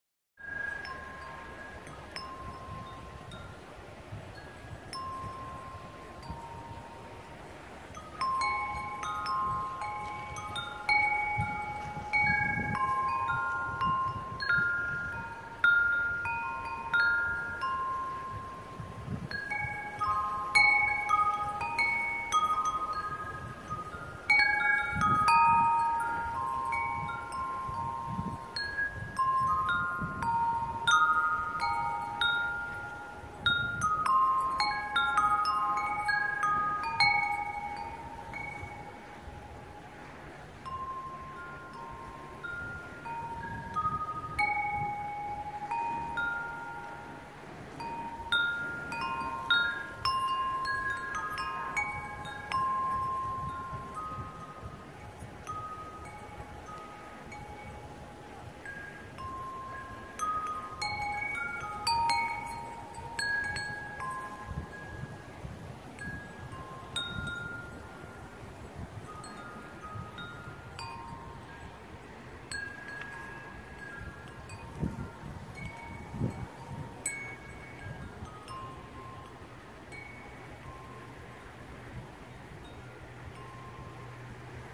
Wind chimes in the Mediterranean.
Video shot in the island of Procida (Gulf of Naples)